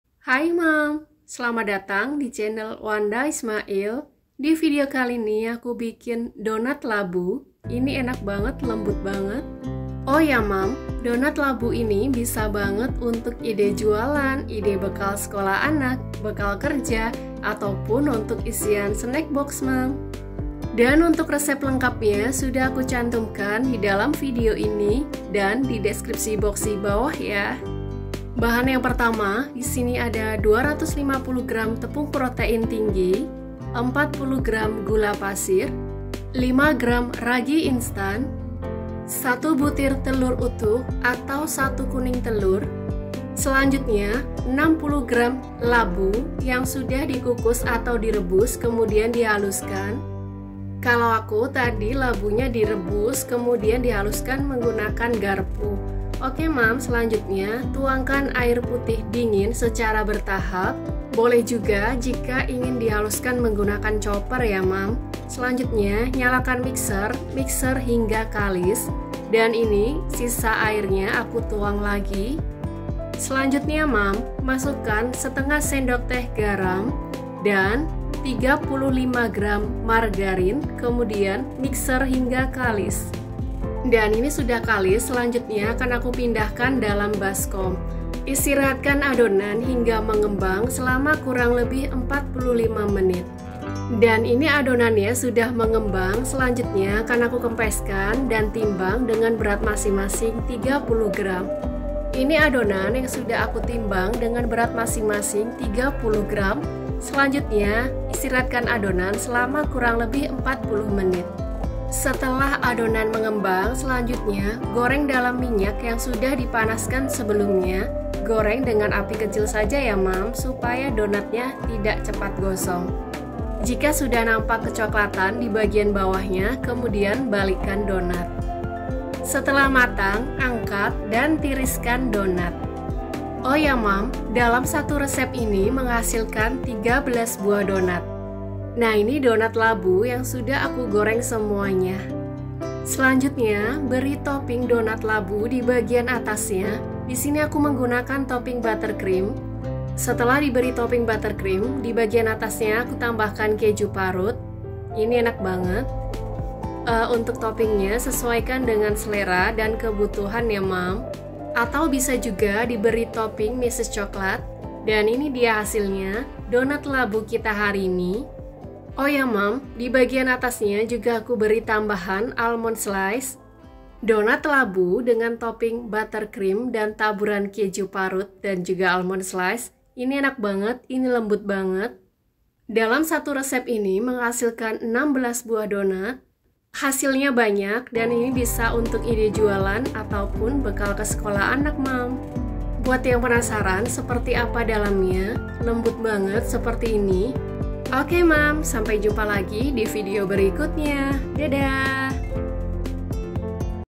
Ide Jualan Donat Labu Lembut Banget
Ide Jualan Donat Lembut Banget
Bahan - bahan :

250 tepung terigu protein tinggi
40 gr gula pasir
5 gr ragi instan
1 butir telur
100 ml air dingin
50 gr labu, rebus atau kukus, haluskan
35gr margarin

Bahan Topping :
buttercream
keju parut
almond slice